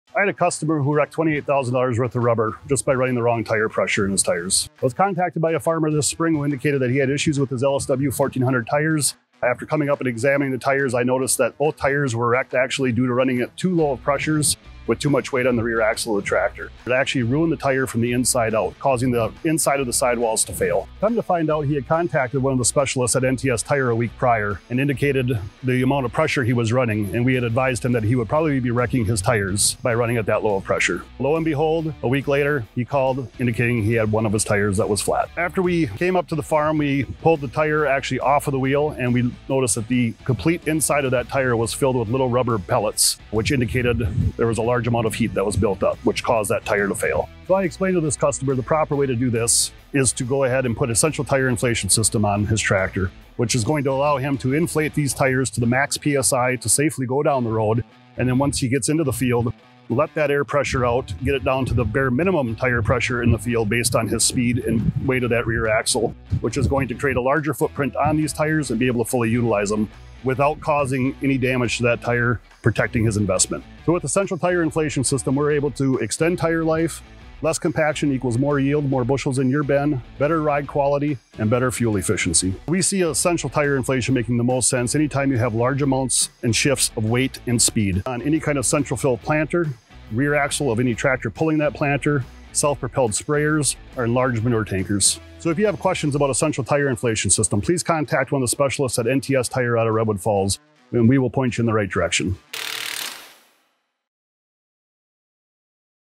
$28,000 Worth of Rubber RUINED!!
This farmer ran the wrong tire pressure and ended up ruining $28,000 worth of tires! Here's how we fixed his situation so he can run low pressure without ruining another set of new tires.